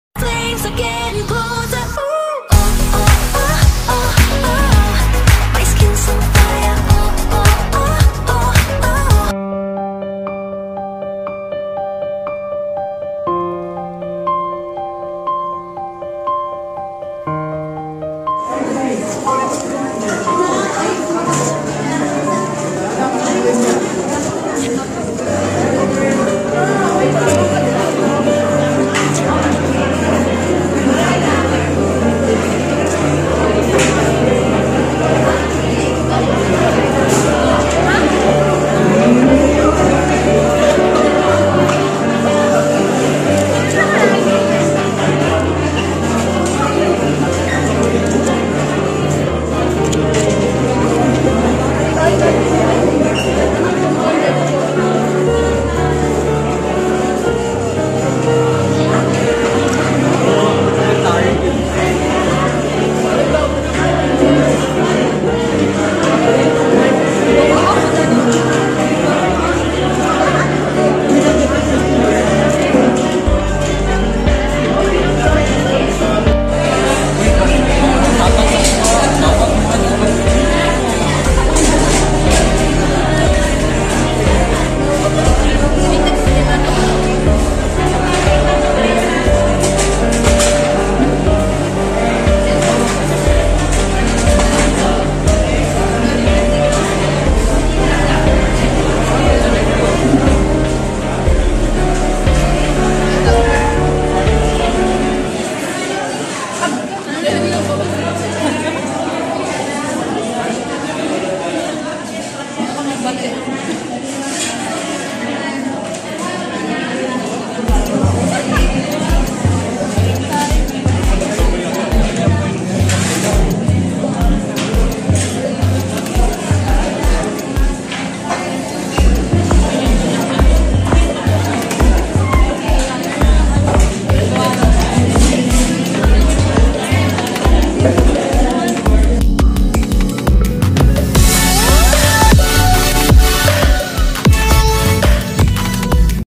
Qah halohalo
Eating halohalo its so yummy best time eating halohalo is summer time come join me.